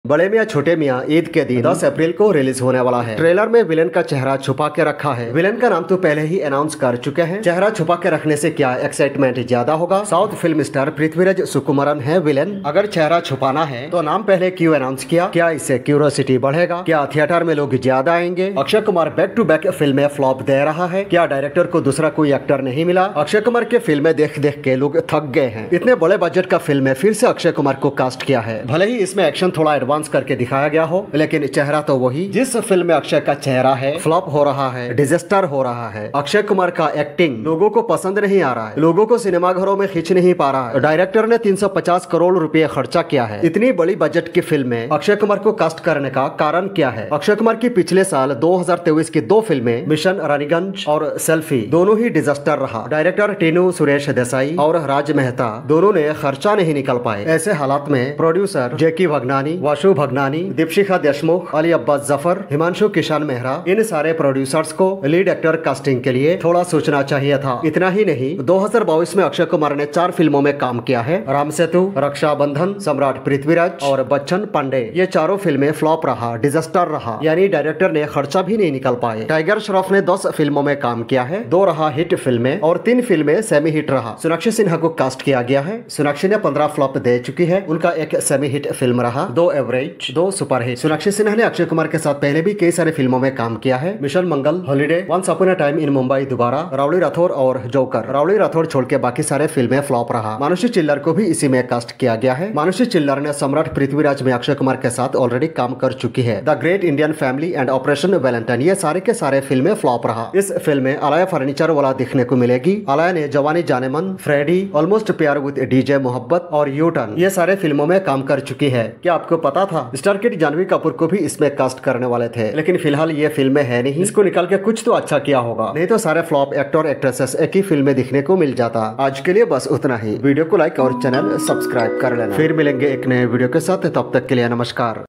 All Flop Actors Casted in Bade Miyan Chote Miyan !! Can This Be a Great Film??
The video is about film Bade Miyan Chote Miyan where Akshay Kumar and Tiger Shroff were casted as lead actor and Suniksha Sinha, Manushi Chiller and Alia Furniture were casted as lead actress.

The video is about the actors and actresses casted in the film.

Thanks!!